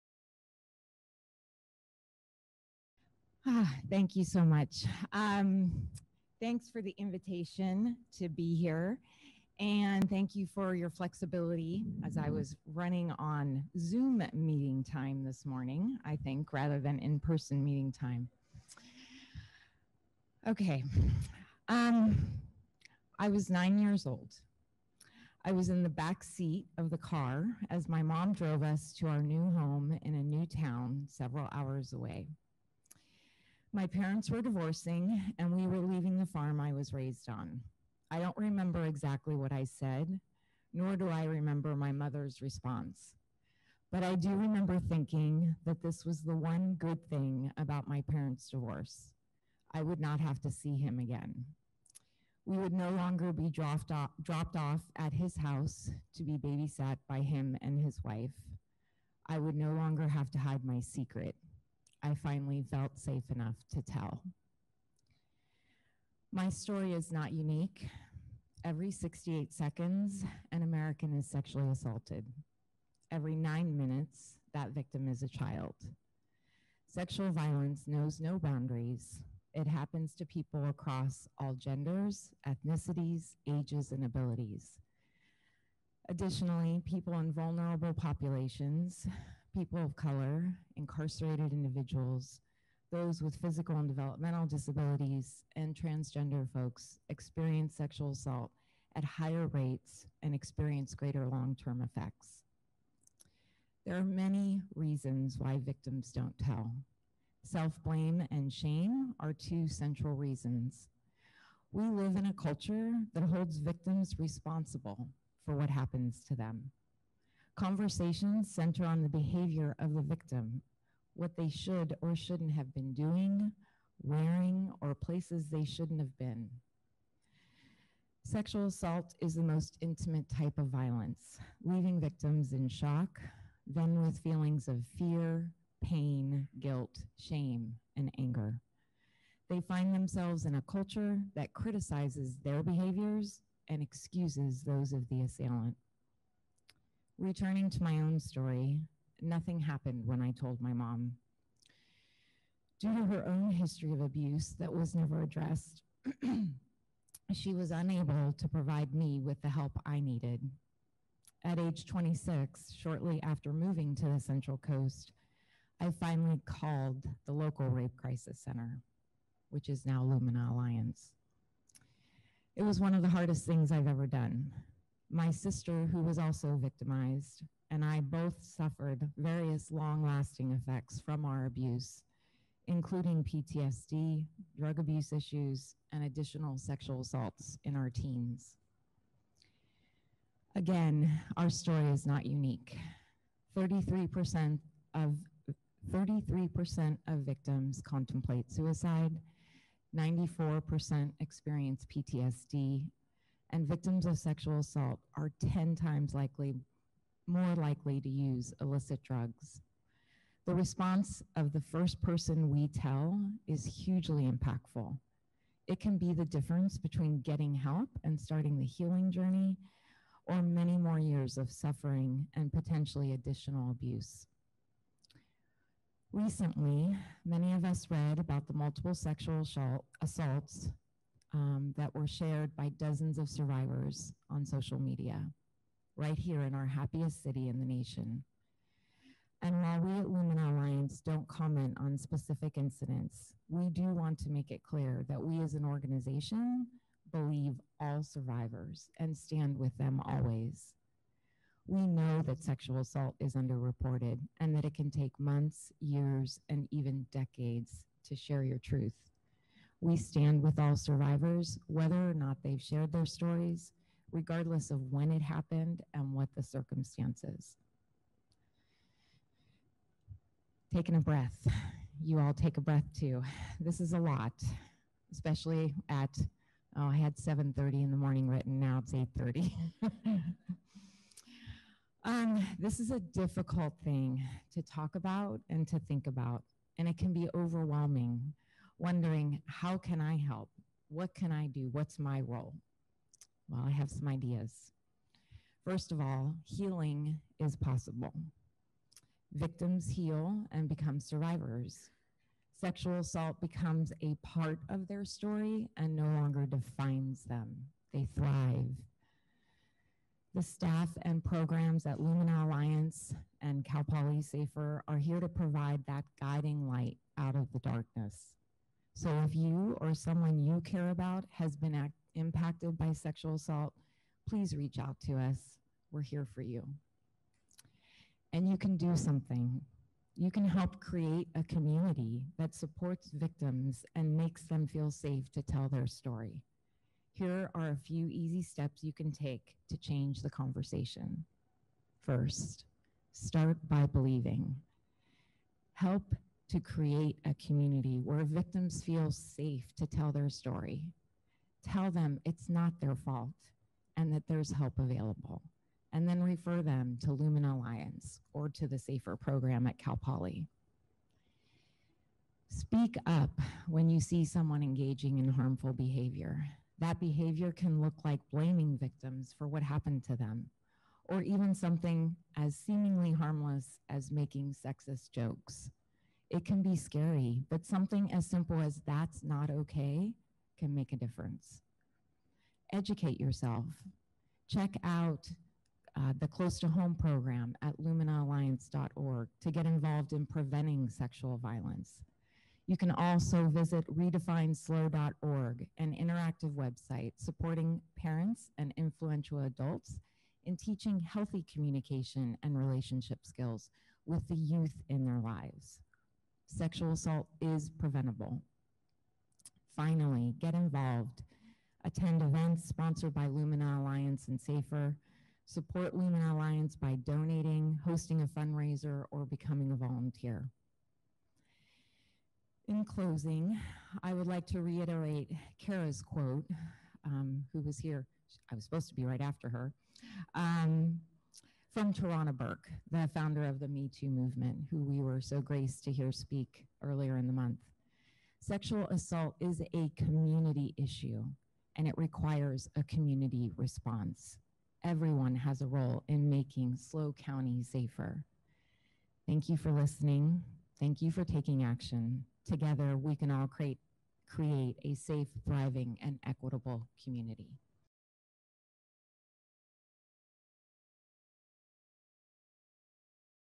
Supporting Survivors | Jennifer Adams
April is Sexual Assault Action Month, and there has never been a more timely moment to take action and support local survivors. Jennifer Adams, Lumina Alliance CEO, sheds light on how to empower survivors in their healing, process sexual violence on a community level, and take steps this month and every month to create a future without violence.